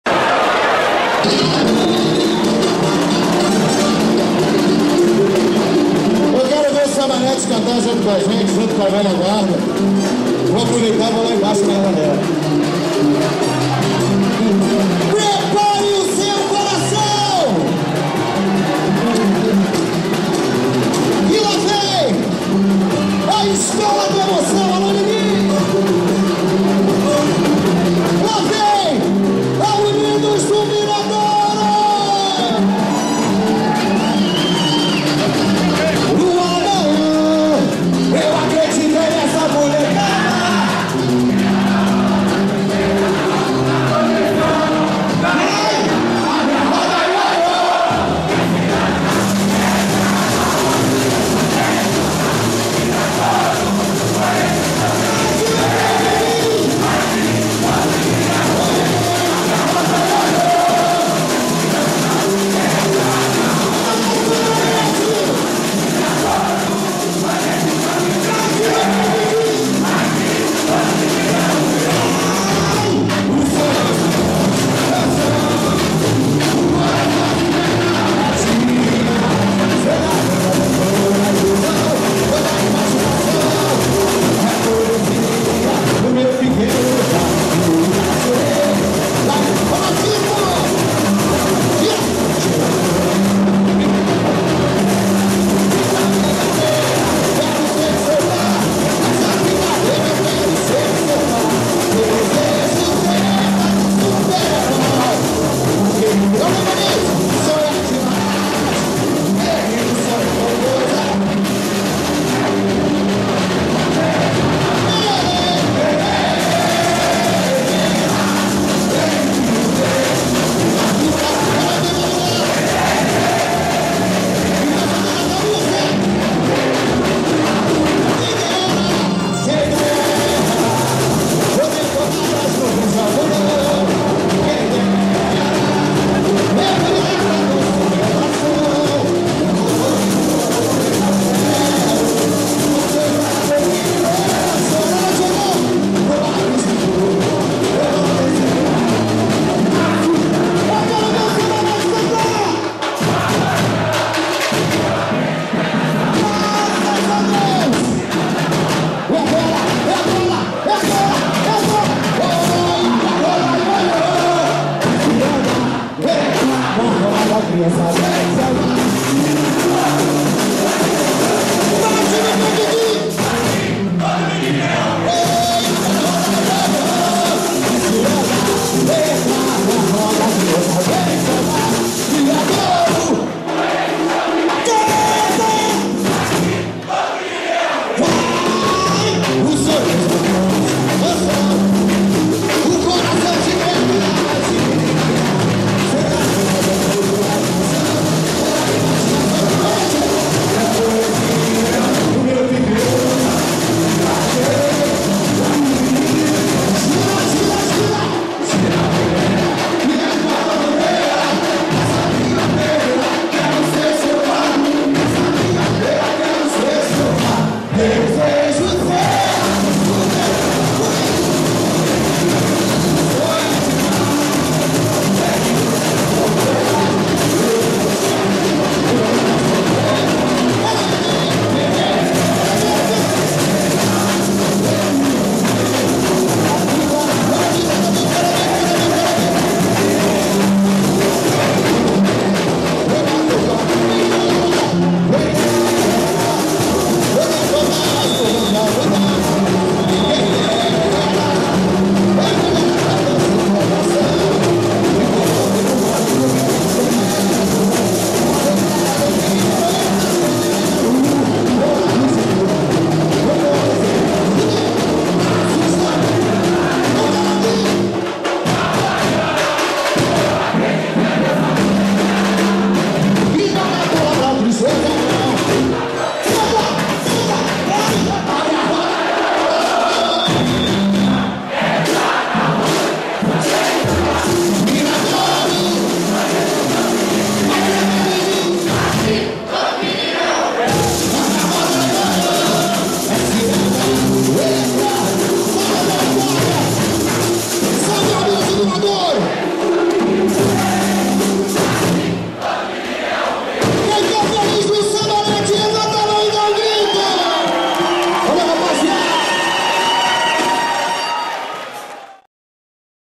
S@mba-net 2017 - Unidos do Viradouro (20/05/2017)
19ª Edição do Prêmio S@mba-net - Carnaval 2017
Apresentação da Unidos do Viradouro
"Mais Elegante Galeria de Velha-Guarda - Série A"
"Mestre Pablo (Personalidade)"
"Jorge Silveira (Revelação)"